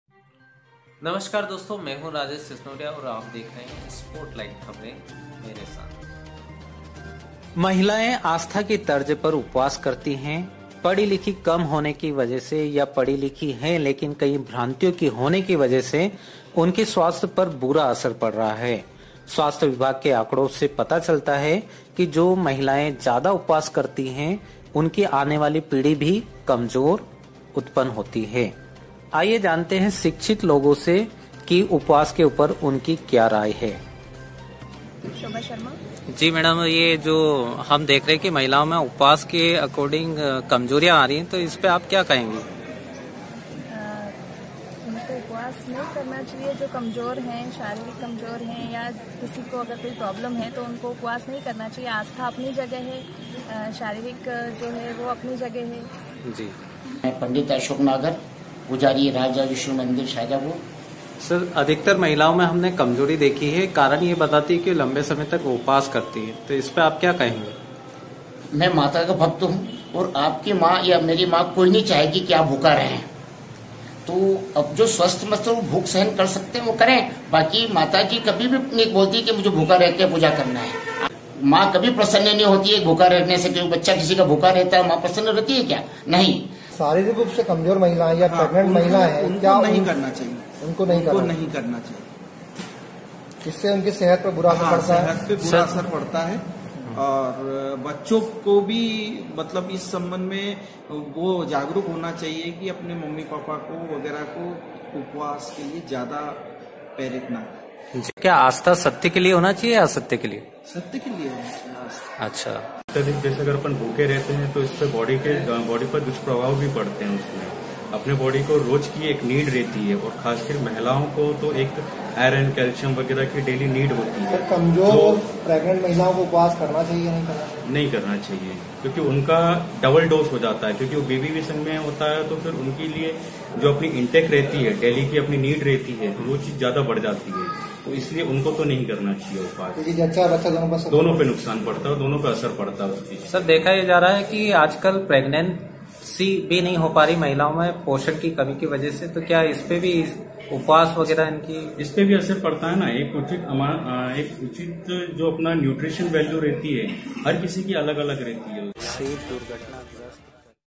Shajapur, उपवास के साइड इफेक्ट्स, पंडित श्री नागर द्वारा, प्रोफेसर और डॉ विशेषज्ञ द्वारा,
उपवास करने और नही करने के फायदे और नुकसान